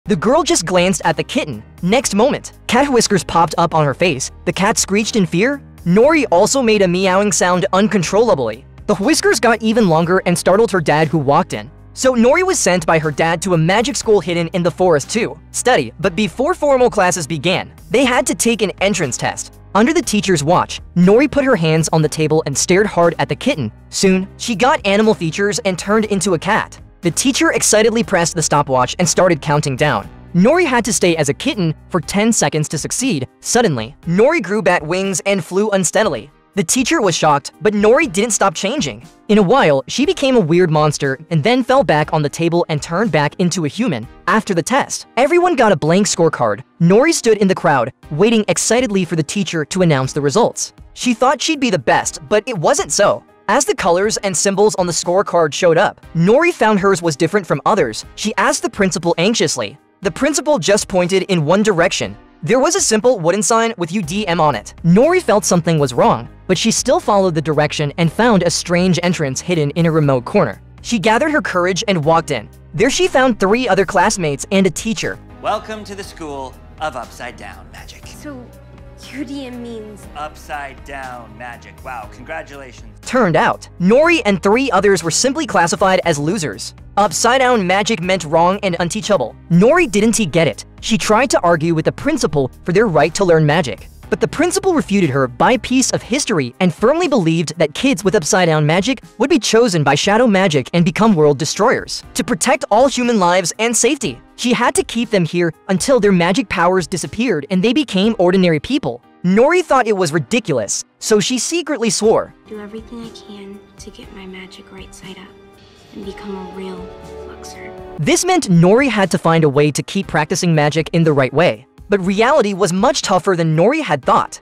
Girl can turn into any animal, but her magic is seen as a failure
Title:
Overview: